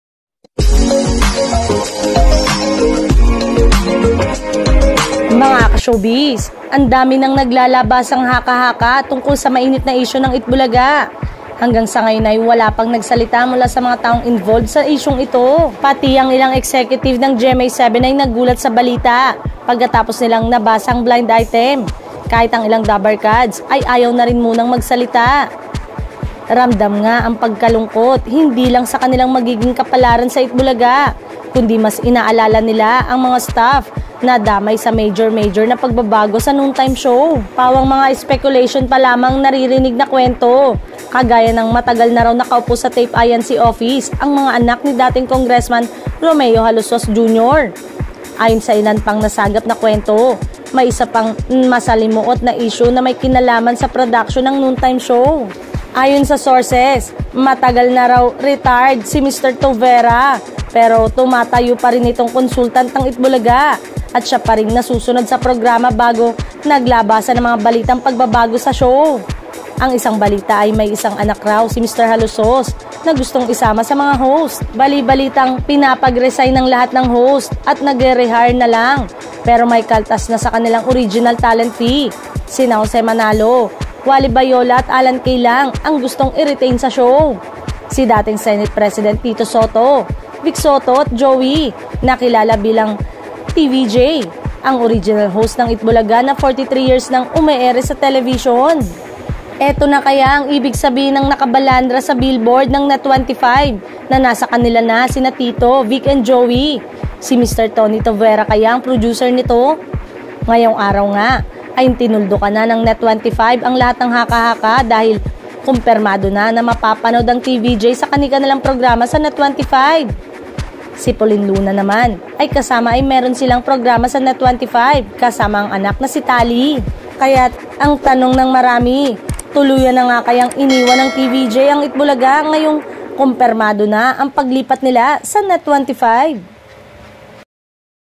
CONFIRMED! TITO, VIC & JOEY NASA NET 25 NA, TULUYAN NG INIWAN ANG EAT BULAGA
Ang dami nang naglalabasang haka-haka tungkol sa mainit na isyu ng Eat Bulaga.

Hanggang sa ngayon ay wala pang nagsalita mula sa mga taong involved sa isyung ito.